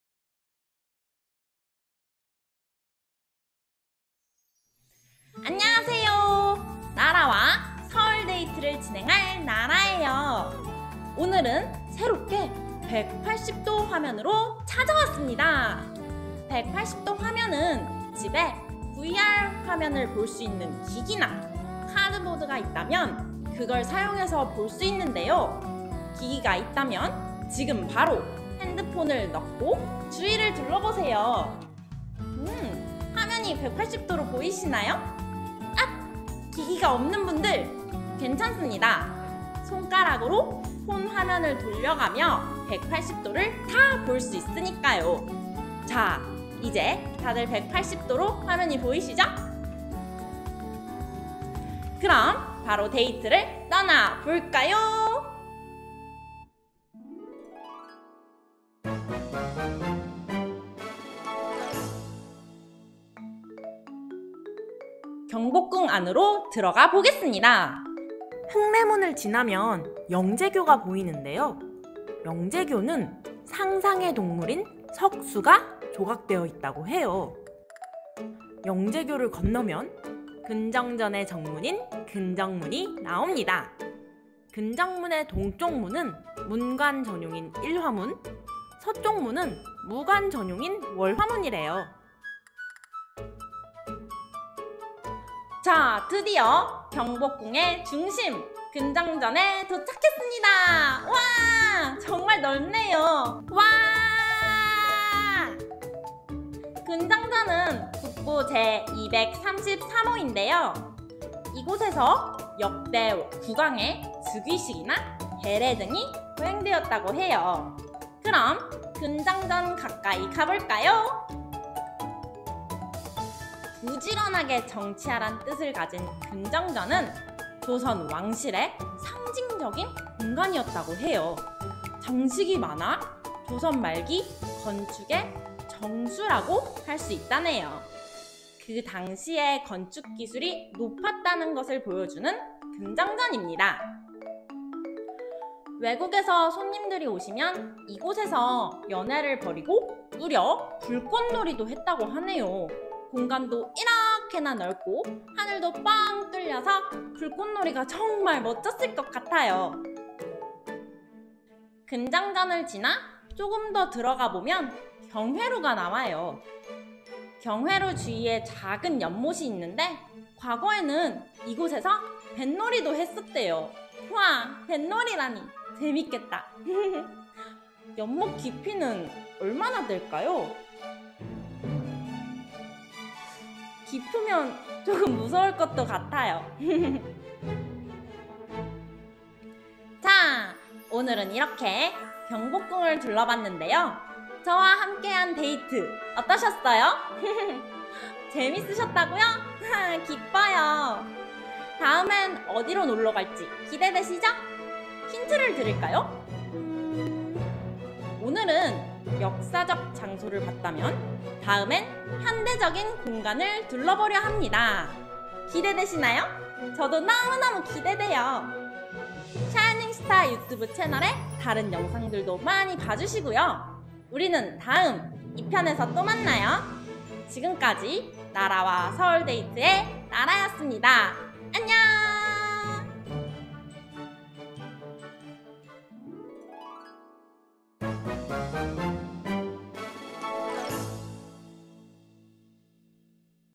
[채나라] 나라랑 서울 데이트 할래? 광화문편 l 샤이닝스타shiningstar
▲ 라이브 일정 : 매주 화요일 오후 4시

하모니 여러분 오늘은 옛날에~~~ 촬영한 영상인데 못 올리고 있다가 이번에 올리게 됐어요!!
그래서 과거의! 나라가 나올 거예요!! 댓글에 가끔씩 찾는 분이 계셔서!
제가 이번에 몰래~ 올리려고 해요~ 모두 쉿! 과거의 나라는 모르게 해주세요!!
이번 주는 과거의! 나라를 만나고! 다음 주부터는 다시 나라로 돌아올게요!
그러면 ~ 안녀어엉!!